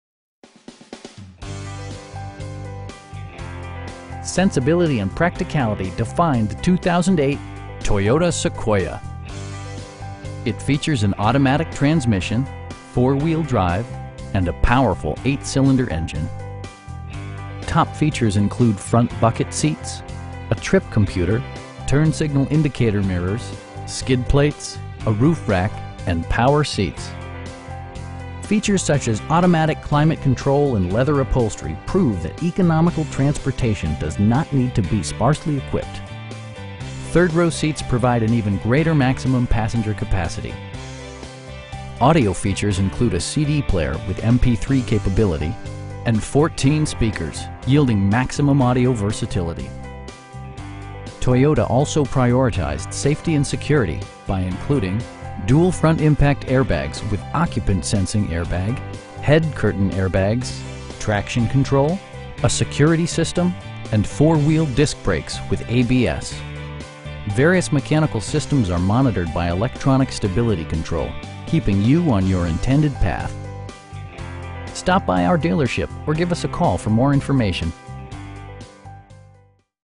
2008 Toyota Sequoia Limited 5.7L V8 in Omaha, NE 68022
Infiniti of Omaha
18201 Burt Street
in Elkhorn, NE 68022

Sensibility and practicality define the 2008 Toyota Sequoia. It features an automatic transmission, 4-wheel drive, and a powerful 8 cylinder engine. Top features include front bucket seats, front and rear reading lights, a trip computer, turn signal indicator mirrors, skid plates, an overhead console, a roof rack, and power seats. Features such as automatic climate control and leather upholstery prove that economical transportation does not need to be sparsely equipped. Third row seats provide an even greater maximum passenger capacity. Audio features include a CD player with MP3 capability, steering wheel mounted audio controls, and 14 speakers, providing excellent sound throughout the cabin. Toyota also prioritized safety and security by including: dual front impact airbags with occupant sensing airbag, head curtain airbags, traction control, brake assist, a security system, and 4 wheel disc brakes with ABS. Various mechanical systems are monitored by electronic stability control, keeping you on your intended path. A Carfax history report provides you peace of mind by detailing information related to past owners and service records. We pride ourselves in consistently exceeding our customer's expectations. Stop by our dealership or give us a call for more information.